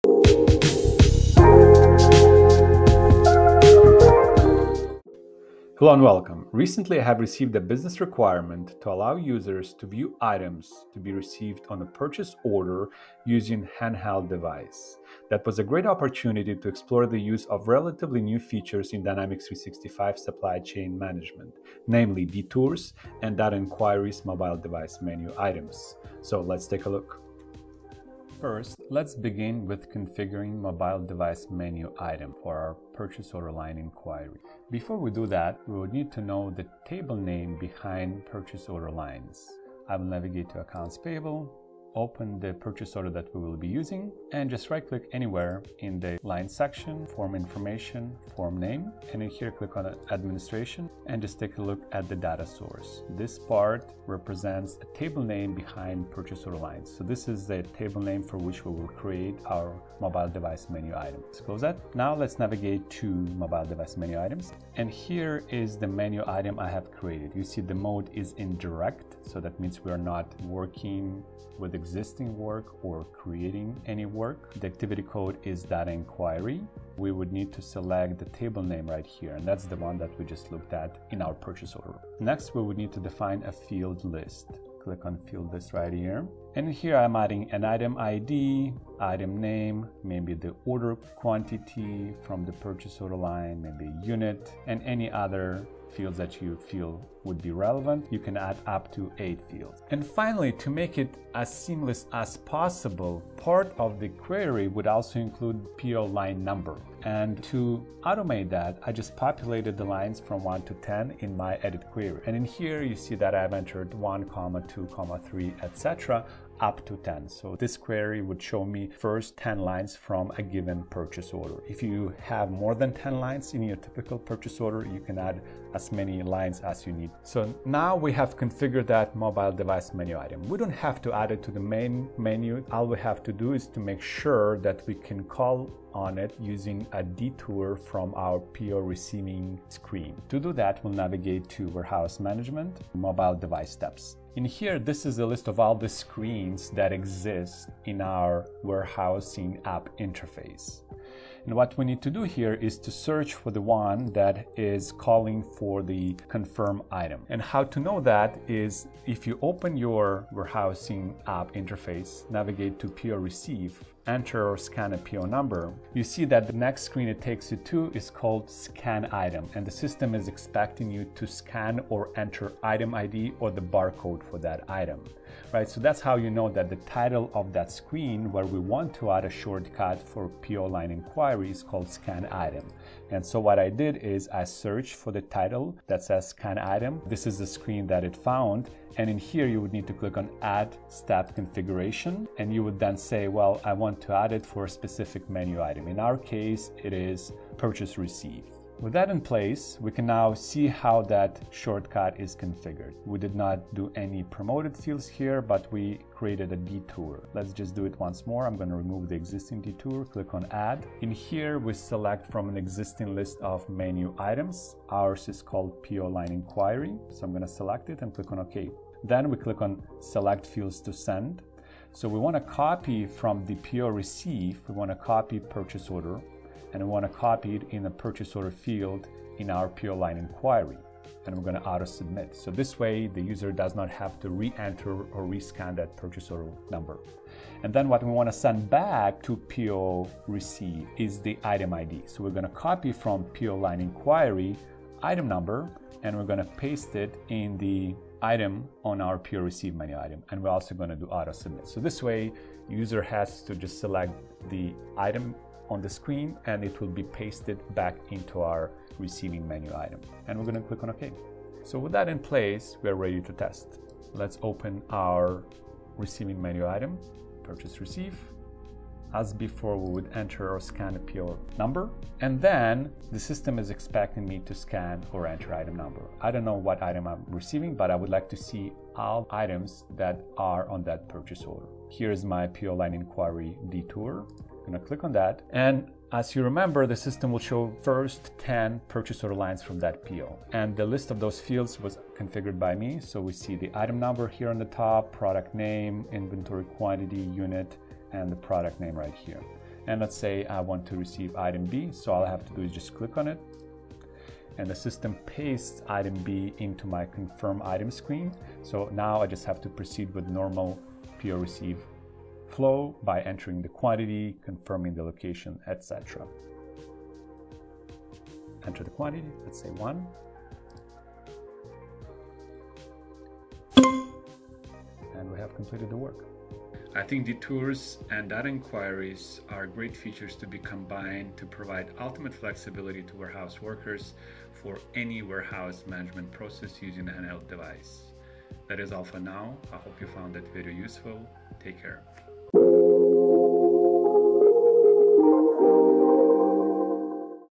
Simplify purchase order receiving with detours and data inquiries - Dynamics 365 F&SCM - Oleksiy K
Discover how to effectively manage warehouse data inquiries with Microsoft Dynamics 365 Supply Chain Management. In this comprehensive tutorial, we delve into the powerful features of the Warehouse App Data Inquiry module. Learn how to streamline your warehouse operations by leveraging real-time data visualization, efficient querying, and insightful analytics. Whether you're a supply chain professional, warehouse manager, or business owner, this video guides you through the process of harnessing the full potential of Dynamics 365 to optimize inventory tracking, enhance decision-making, and ensure seamless logistics. Watch now to unlock the secrets of data-driven warehouse management and elevate your business to new heights of efficiency and success.